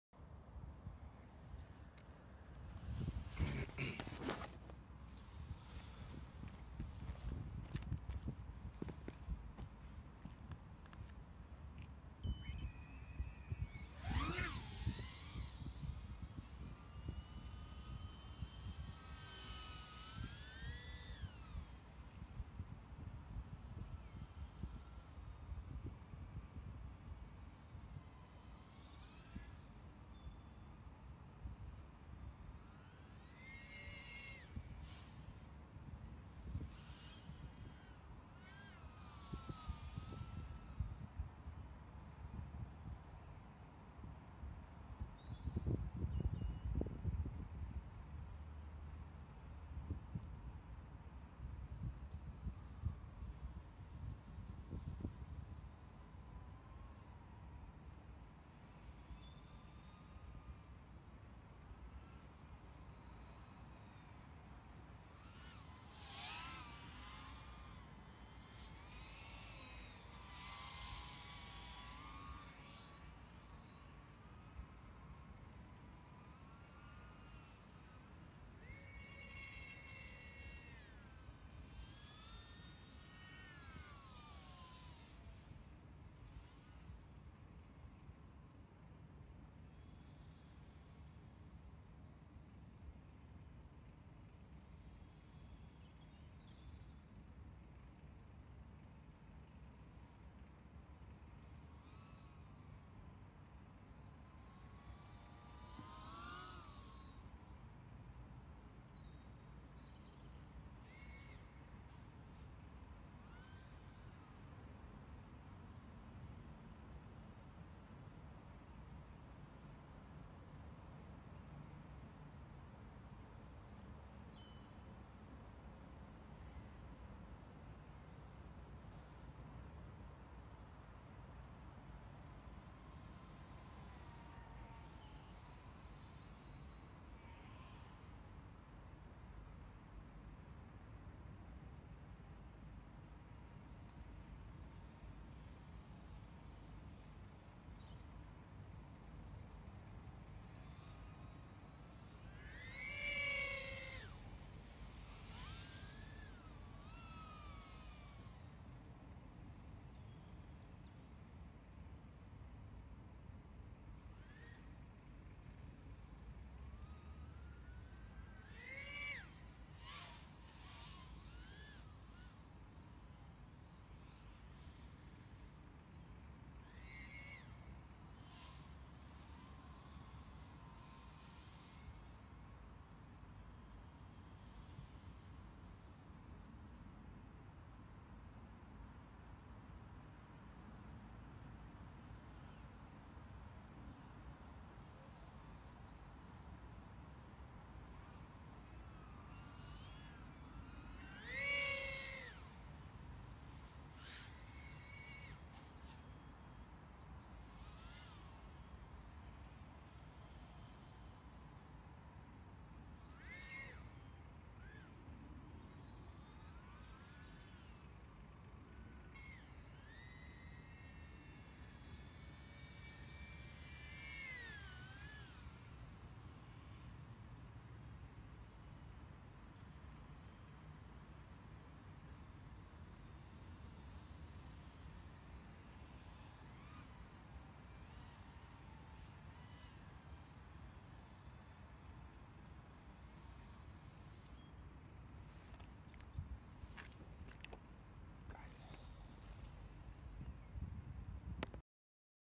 GEPRC Sparrow FPV filmed with Eachine ROTG01
GEPRC MX3 Sparrow
HGLRC XJB Mini F425 Tower
1407 Brother Hobby
GR12L SUMD
Eachine ATX03
Foxeer Mini V2 mit OSD
850mah 4s mylipo